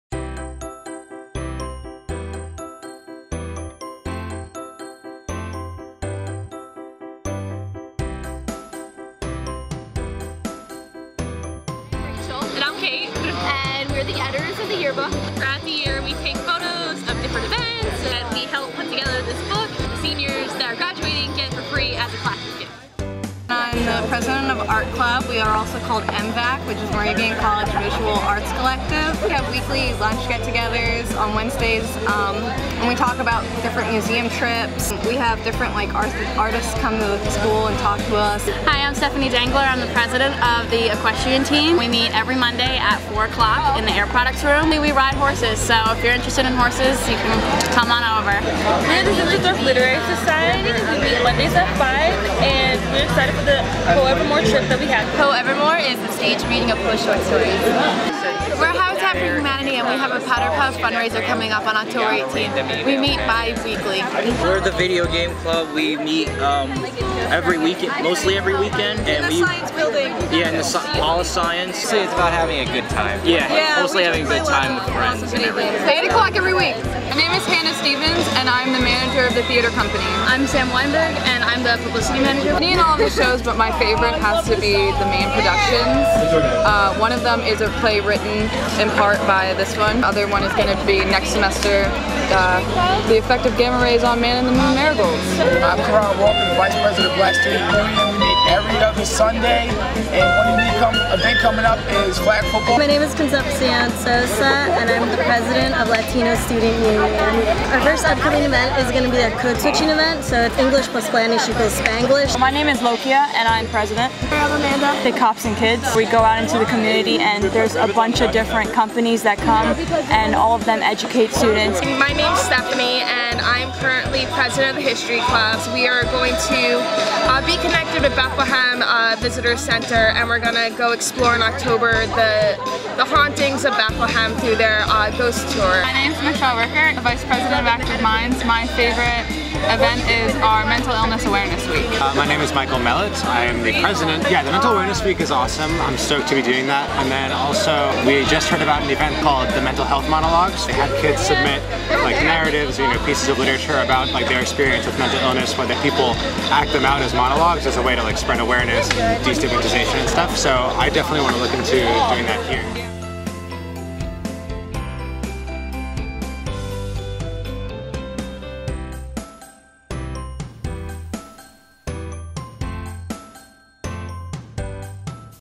Clubs and Organizations at Moravian College
Some of the many clubs and organizations that Moravian College has to offer.